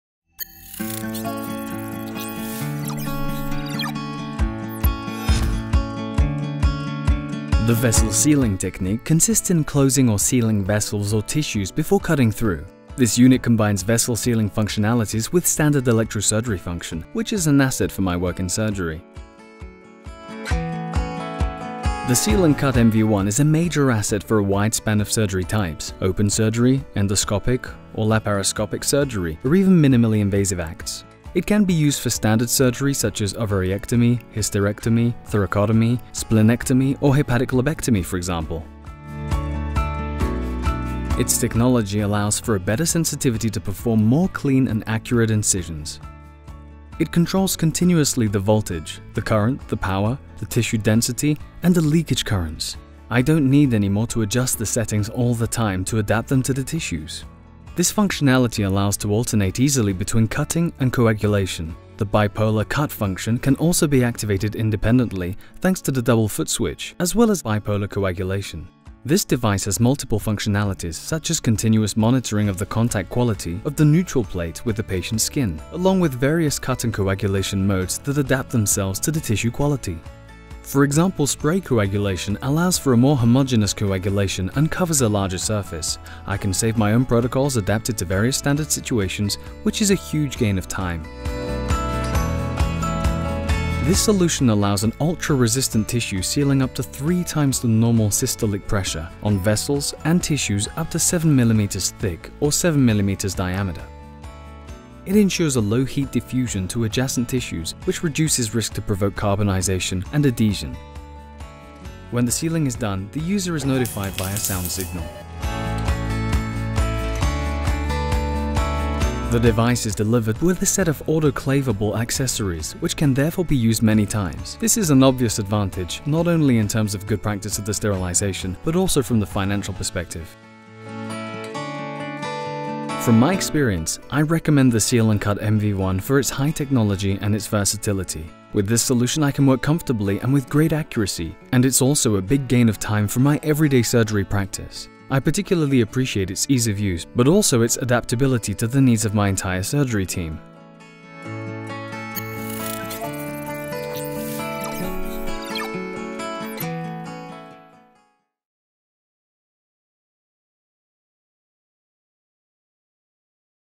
Seal&Cut MV1, the VETERINARY vessel sealing by Mano Médical
Make your surgeries simpler, quicker and safer!
🔹 Vessel sealing, monopolar and bipolar ESU all-in-one: save space in your OR
🔹 Reusable and autoclavable accessories: huge savings and high standard sterilization
🔹 Smart access to presets: save your time
🔹 Advanced technology: reduces side damage to tissues while ensuring precise cutting